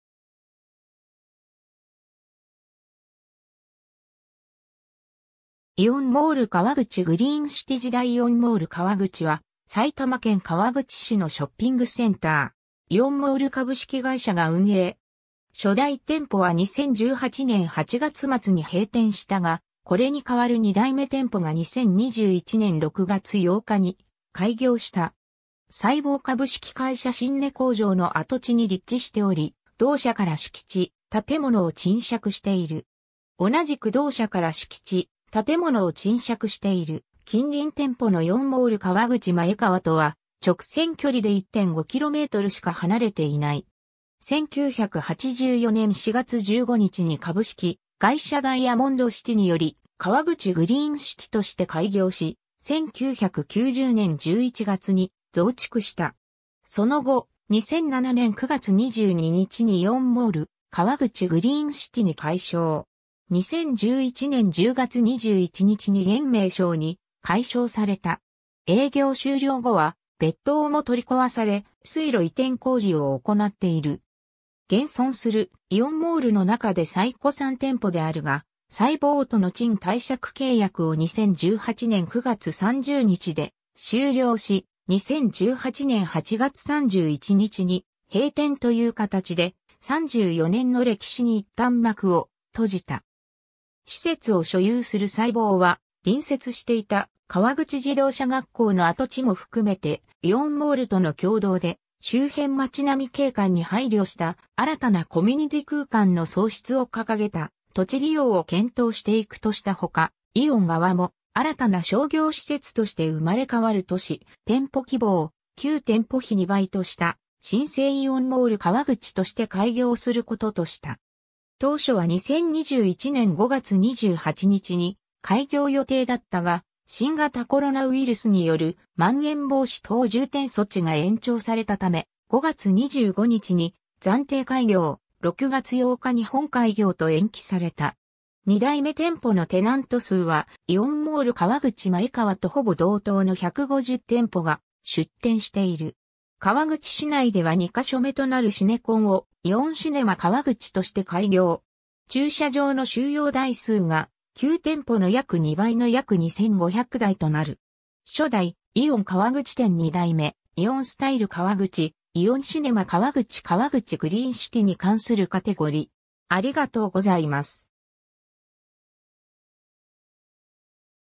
イオンモール川口
イオンモール川口グリーンシティ時代 イオンモール川口（イオンモールかわぐち、AEON MALL KAWAGUCHI）は、埼玉県川口市のショッピングセンター。
イオンモール株式会社が運営。
初代店舗は2018年8月末に閉店したが、これに代わる2代目店舗が2021年6月8日に開業した。
 サイボー株式会社神根工場（1954年2月から1982年7月まで操業）の跡地に立地しており、同社から敷地・建物を賃借している。
同じく同社から敷地・建物を賃借している近隣店舗のイオンモール川口前川とは、直線距離で1.5kmしか離れていない。
1984年（昭和59年）4月15日に株式会社ダイヤモンドシティにより川口グリーンシティとして開業し、1990年（平成2年）11月に増築した。
 その後、2007年（平成19年）9月22日にイオンモール川口グリーンシティに改称。
2011年（平成23年）10月21日に現名称に改称された。
営業終了後は別棟（開業当初は自動車販売店が入店していたが、初代店舗の閉店時はテナントを入店させていた）も取り壊され、水路移転工事を行っている。
 現存するイオンモールのなかで最古参店舗であるが、サイボーとの賃貸借契約を2018年（平成30年）9月30日で終了し、2018年8月31日に閉店という形で、34年の歴史に一旦幕を閉じた。
施設を所有するサイボーは、隣接していた川口自動車学校（2013年10月で閉校）の跡地も含めてイオンモールとの共同で「『周辺まちなみ景観に配慮した新たなコミュニティ空間の創出』を掲げた土地利用を検討していく」としたほか、イオン側も「新たな商業施設として生まれ変わる」とし、店舗規模を旧店舗比2倍とした『新生イオンモール川口』として開業する事とした。
当初は2021年（令和3年）5月28日に開業予定だったが、新型コロナウイルスによるまん延防止等重点措置が延長されたため、5月25日に暫定開業、6月8日に本開業と延期された。
 2代目店舗のテナント数は、イオンモール川口前川とほぼ同等の150店舗が出店している。
川口市内では2ヶ所目となるシネコンを「イオンシネマ川口」として開業。
駐車場の収容台数が旧店舗の約2倍の約2500台(平面と立体の駐車場を合算)となる。
 初代：イオン川口店（2011年2月28日まではジャスコ川口店） 2代目：イオンスタイル川口、イオンシネマ川口 川口グリーンシティに関するカテゴリ: